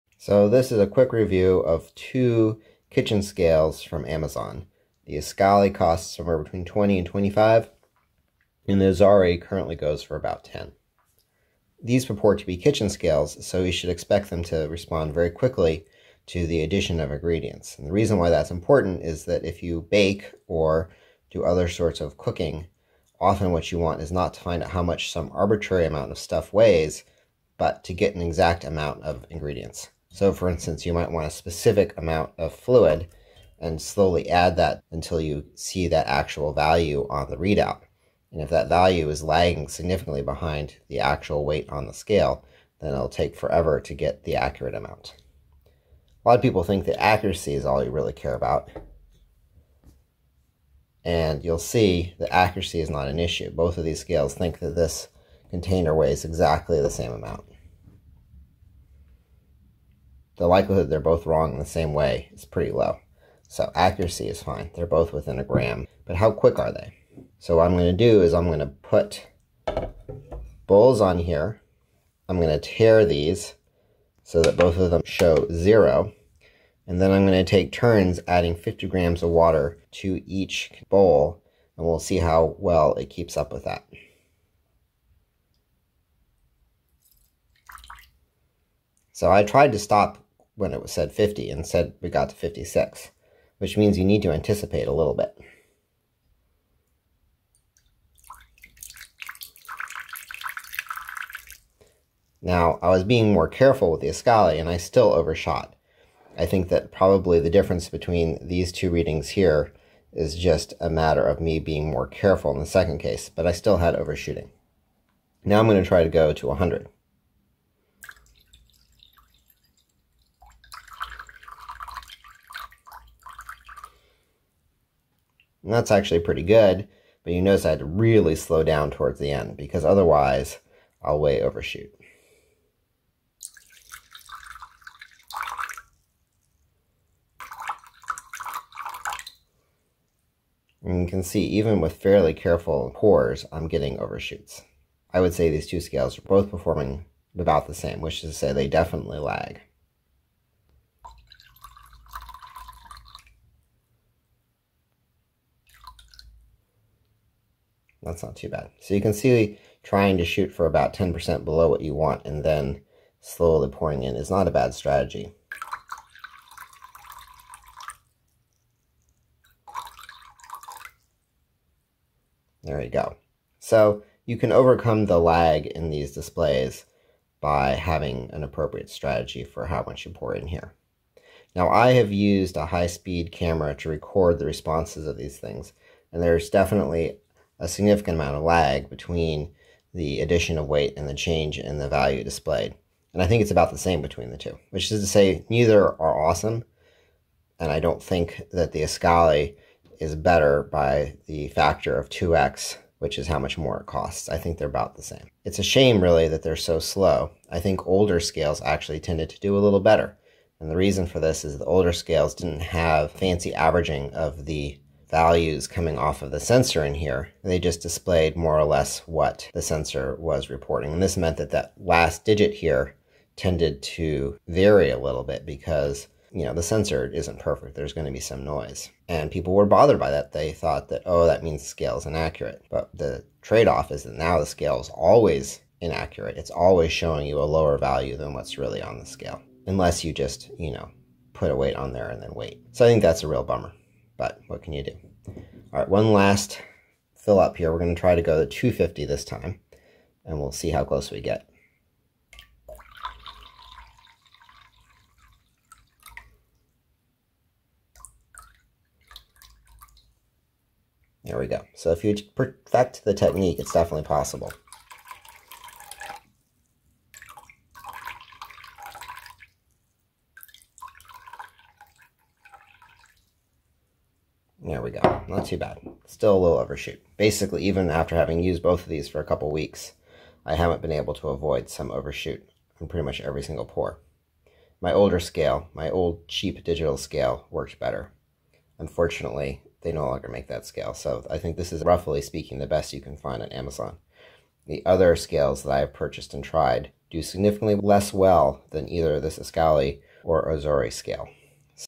Escali Primo Kitchen Scale Vs Ozeri Pronto Kitchen scale review: are they usable for baking?
These two scales are equally accurate for static items, but both respond slowly to changing weight, making them less than ideal for the kitchen. That said, I've seen much slower. This video demonstrates the problem so you can decide if either would be good enough for you.